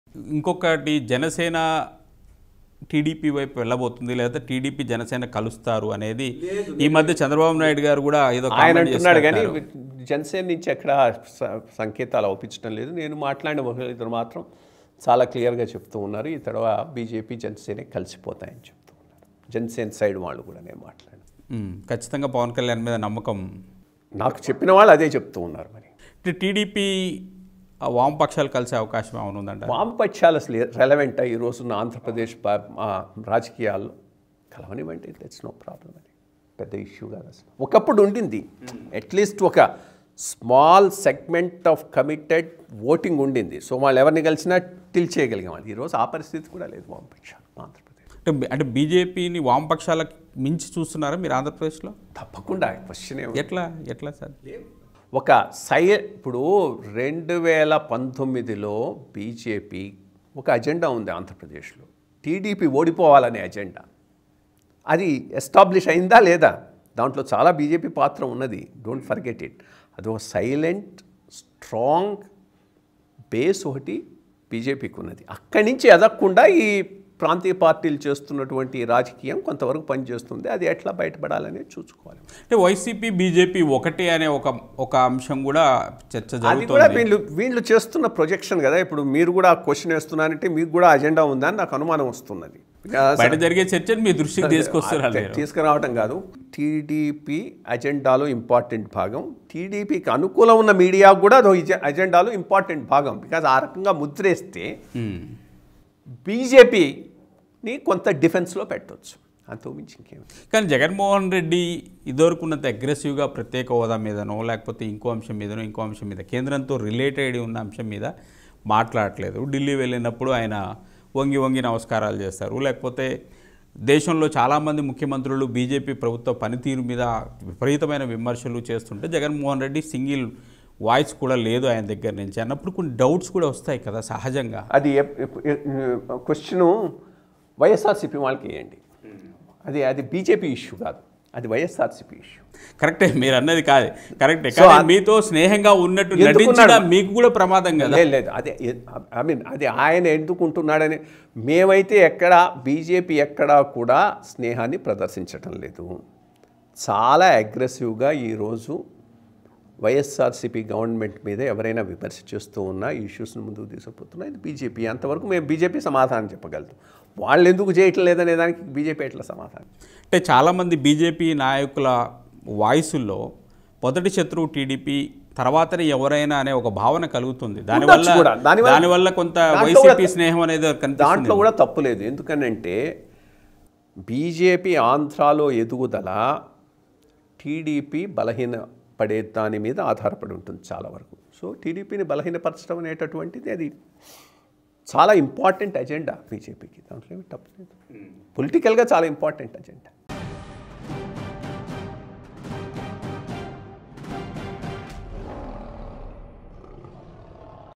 Former AP Chief Secretary IYR Krishna Rao Comments on TDP & Janasena | BJP | Leo Politics
Leo Politics

Leo Health

Leo Entertainment

Leo Devotional